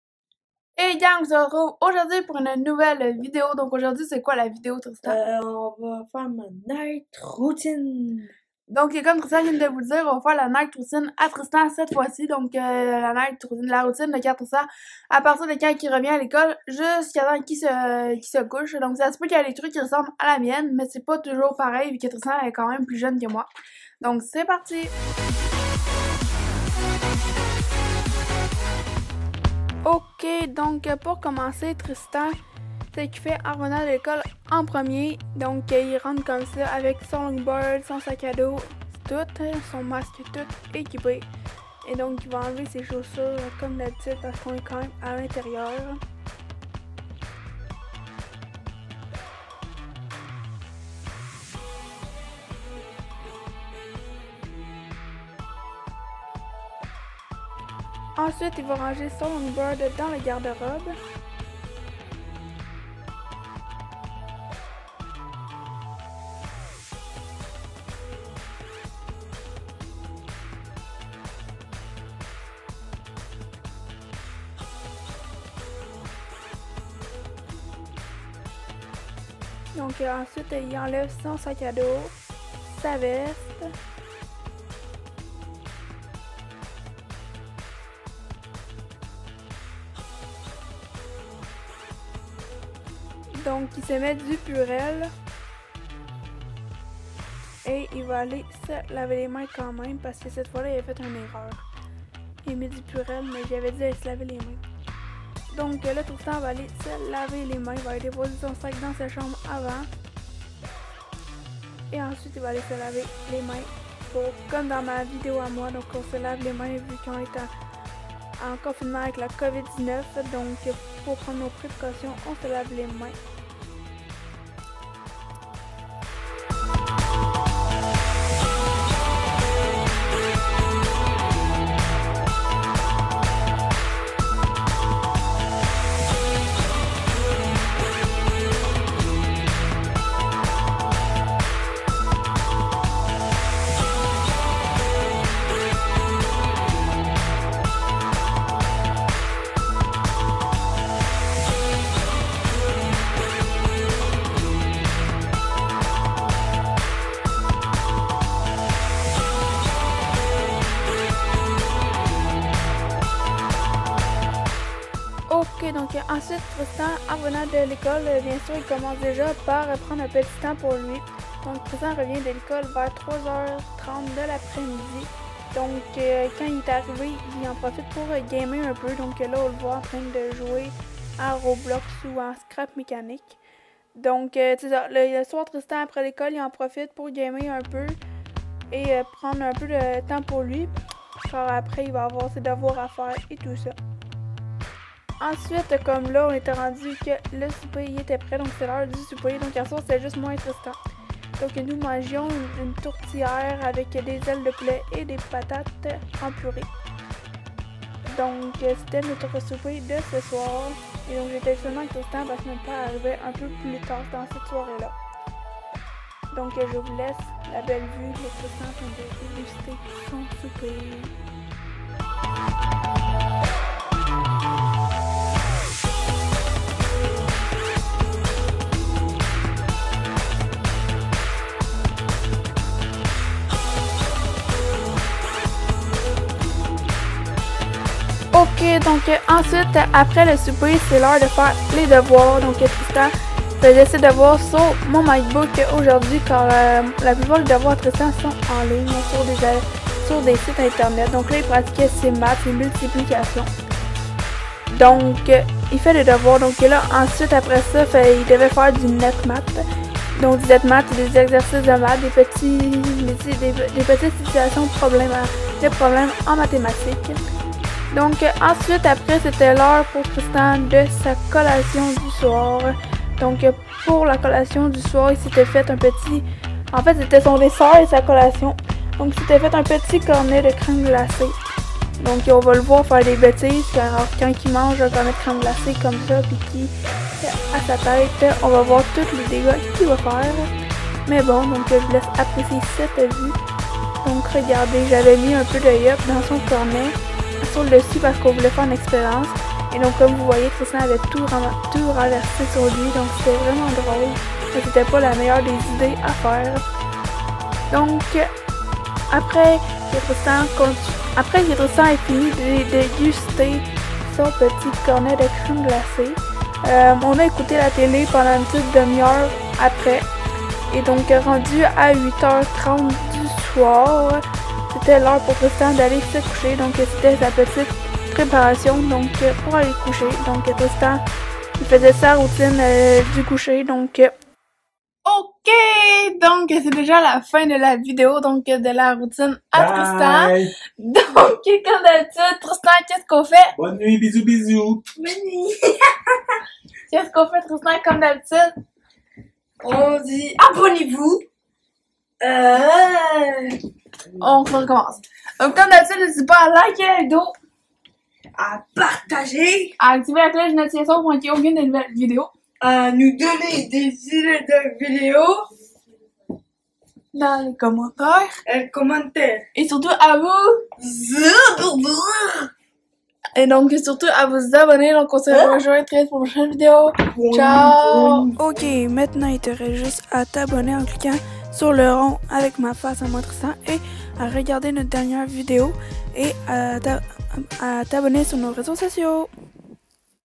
NIGHT ROUTINE DE TRISTAN//Lea Réglisse//
Aujourd'hui, c'est au tour de Tristan de vous montrer sa routine du soir après l'école.

Ps:N'oublie pas de me donner d'autre idée de vidéo dans les commentaires !